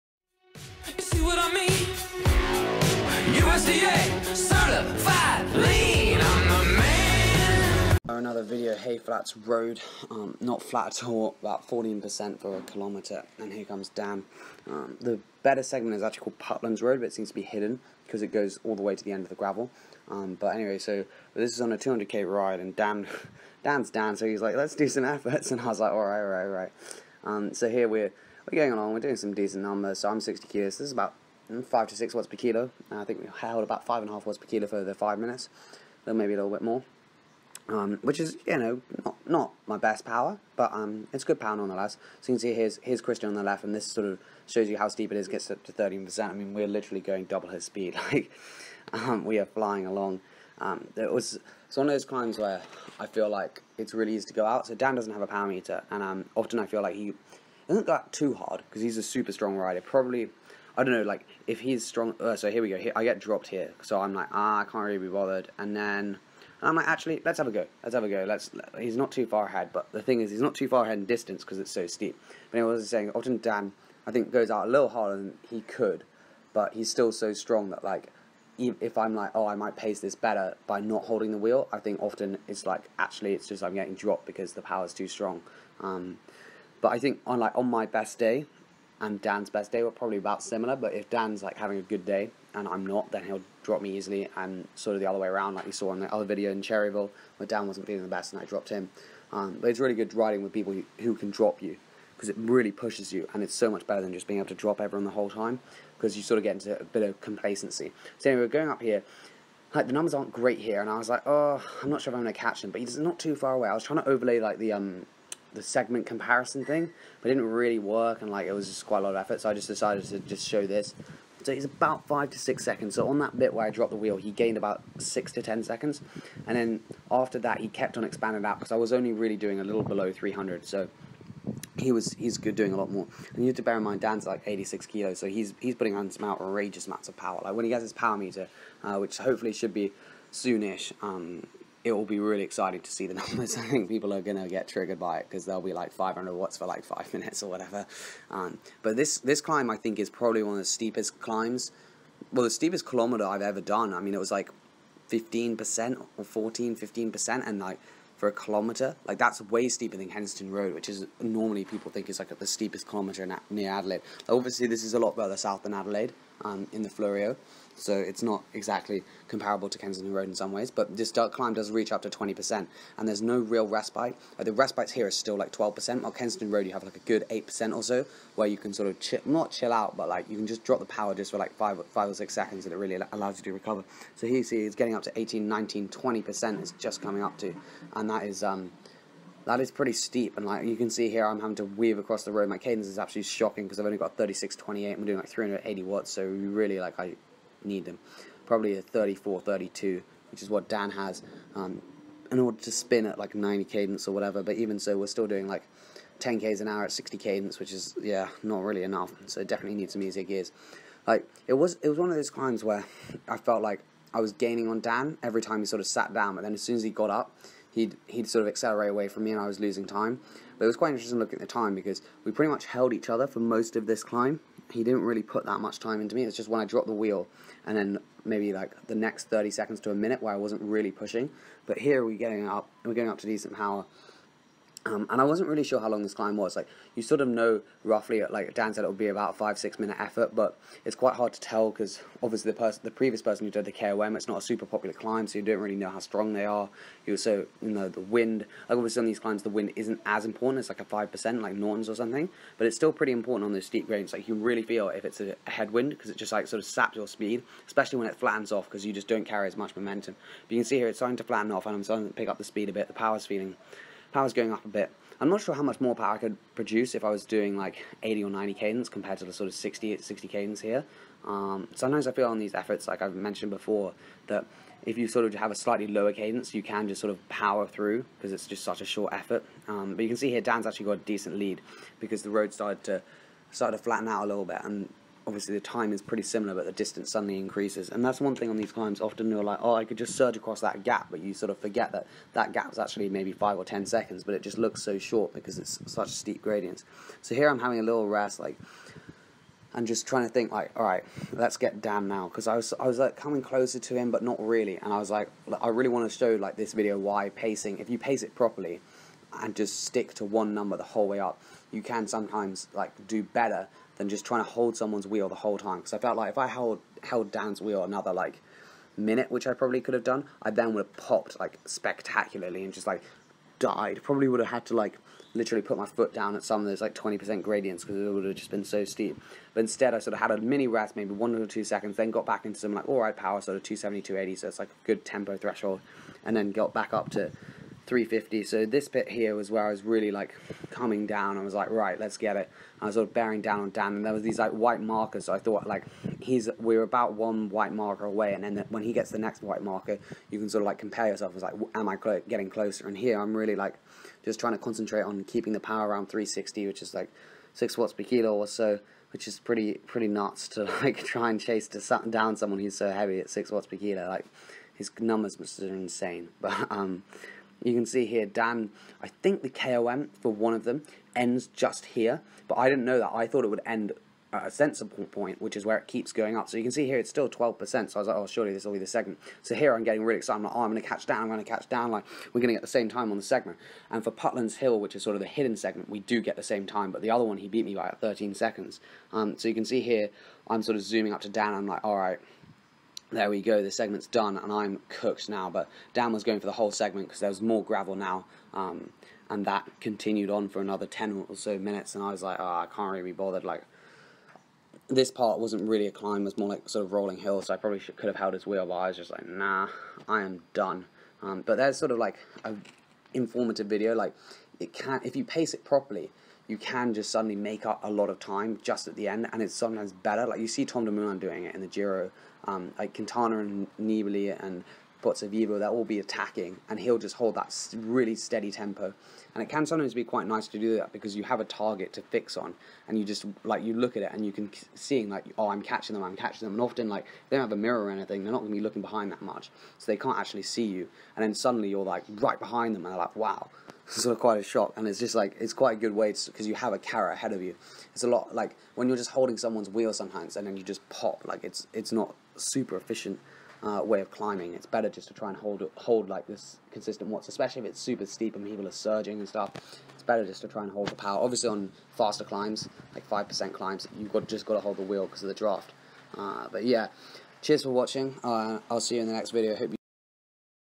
How to Pace yourself on a Climb
Great climb called 'Hay flat road' 14% for 1km. Demonic Dan was really strong and so I let the wheel go but paced it so I came back and finished not too far behind him. Strava